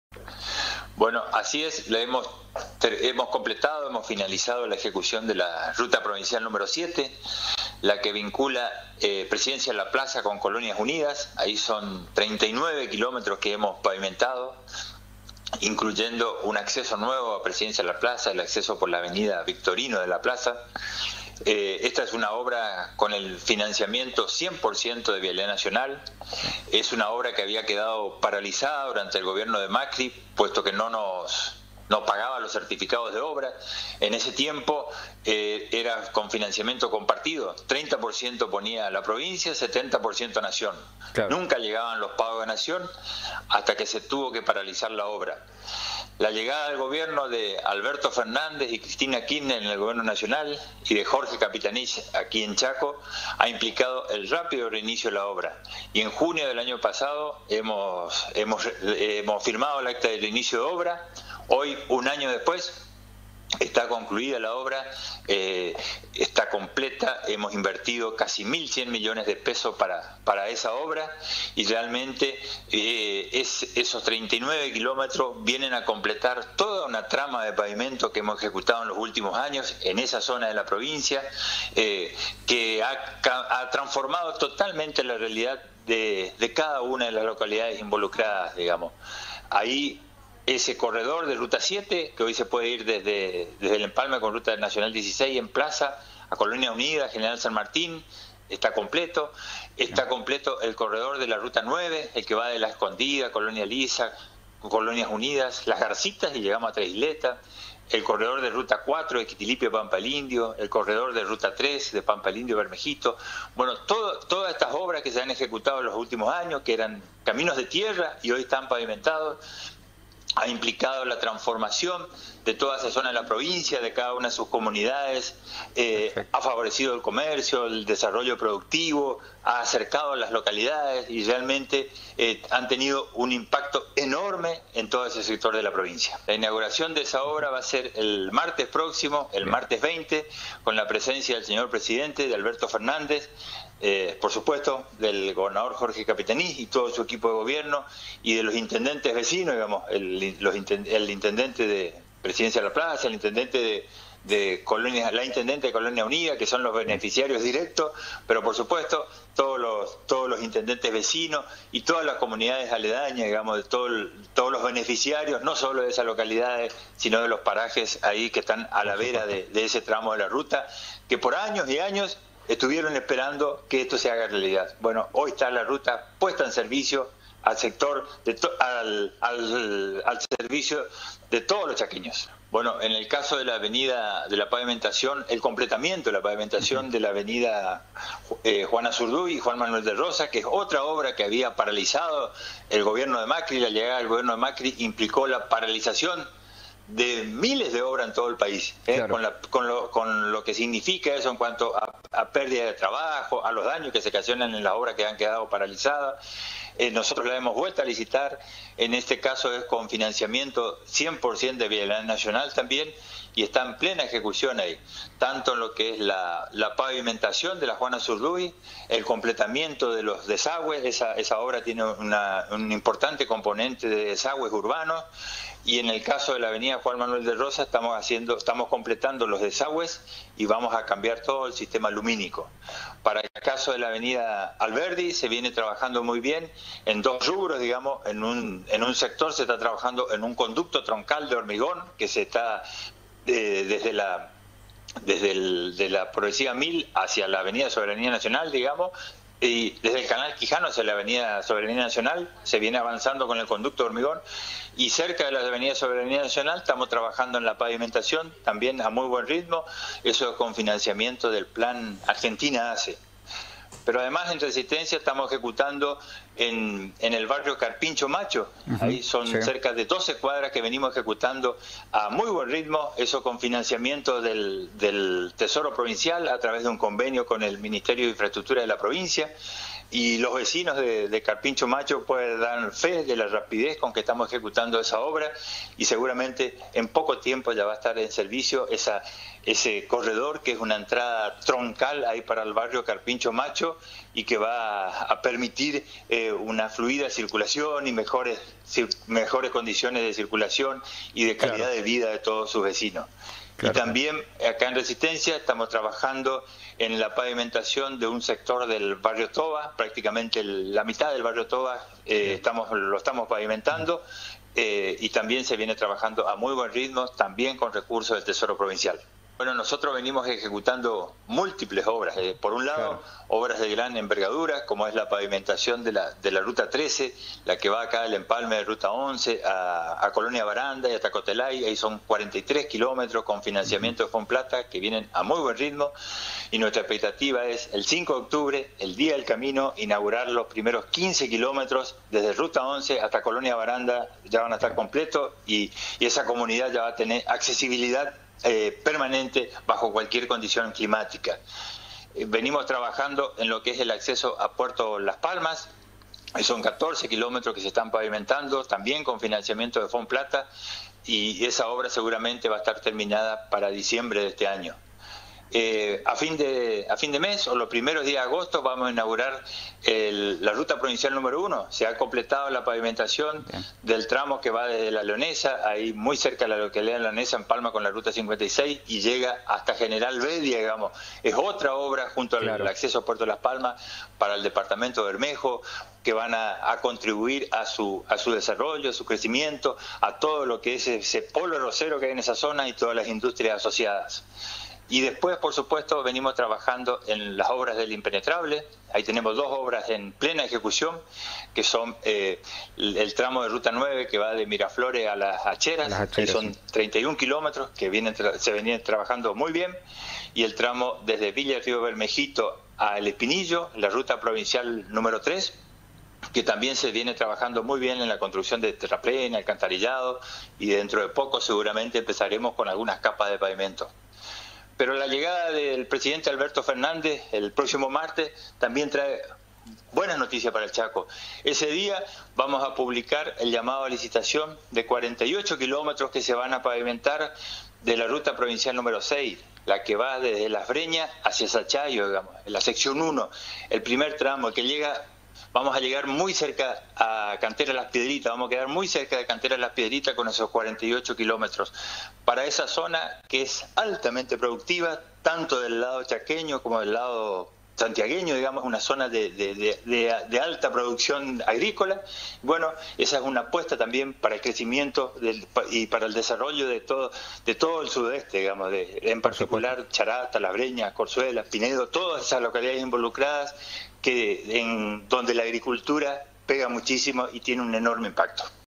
FINALIZÓ LA OBRA DE RUTA 7
HUGO VARELA - ADMINISTRADOR GRAL. DE VIALIDAD PROVINCIAL
Vialidad Provincial concluyó las tareas correspondientes a la obra de pavimentación en la Ruta Prov. Nº 7 en el tramo que va desde Plaza hasta Colonias Unidas. Se espera la presencia del Presidente Alberto Fernández para acompañar al gobernador Jorge Capitanich en el acto inaugural de la semana que viene.